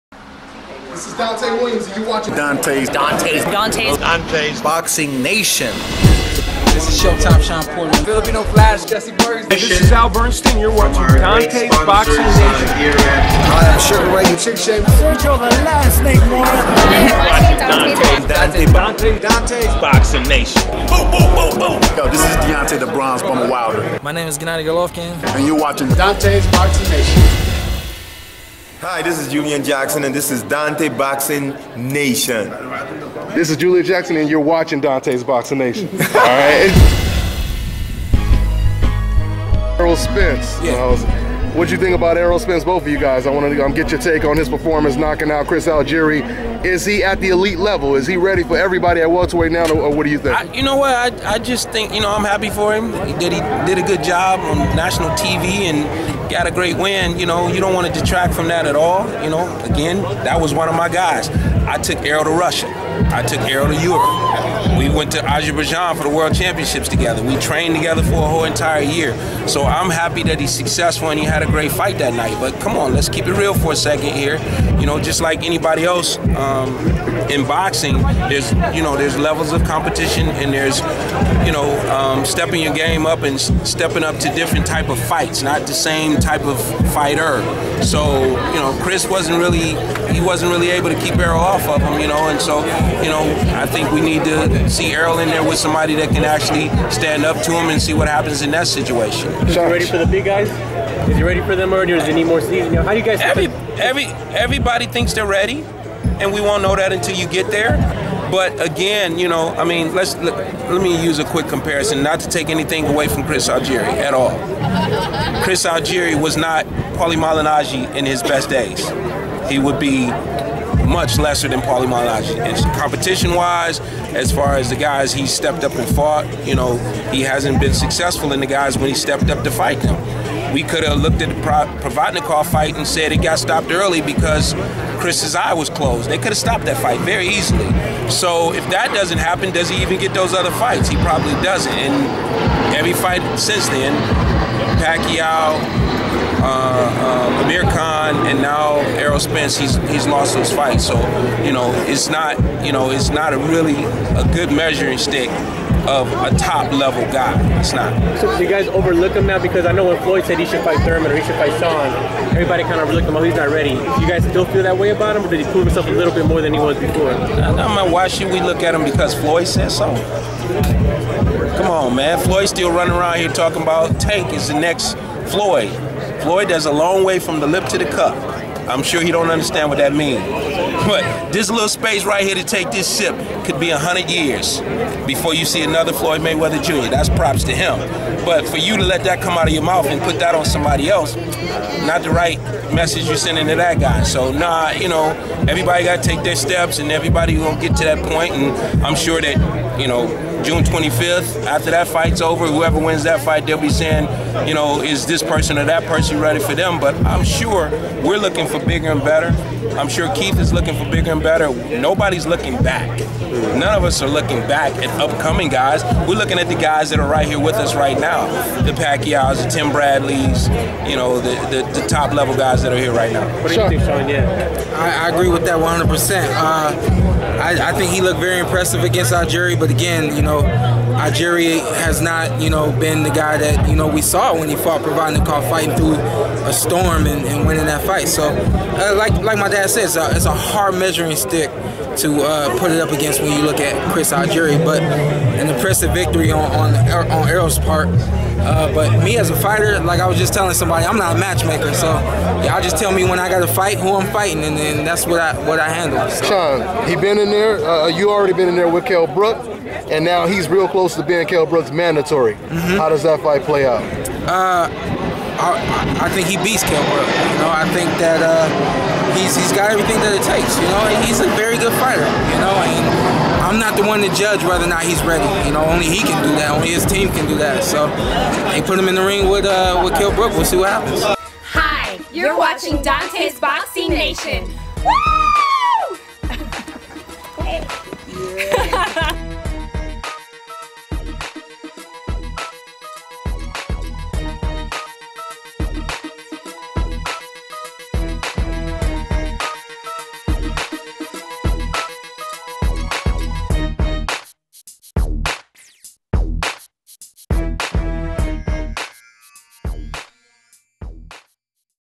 KENNY PORTER SAYS PORTER-MALIGNAGGI WIN WAS BIGGER THAN SPENCE-ALGIERI WIN
Kenny Porter suggest Errol Spence is not on Shawn Porters level.